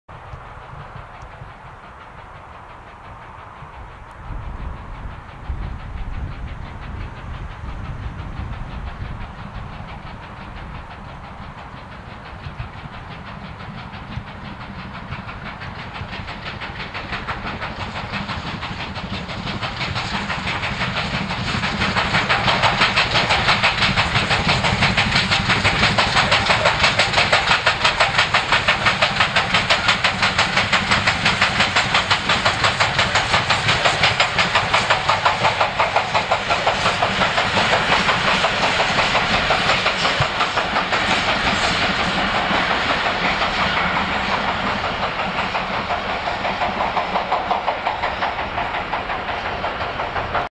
BR 23 042 mit Volldampf zum Rüdesheimer Weihnachtsmarkt 2011
Sonderzug aus Frankfurt/Main über Wiesbaden-Biebrich zum Weihnachtsmarkt der Nationen in Rüdesheim (Zuglok ist 23 042 aus Darmstadt-Kranichstein)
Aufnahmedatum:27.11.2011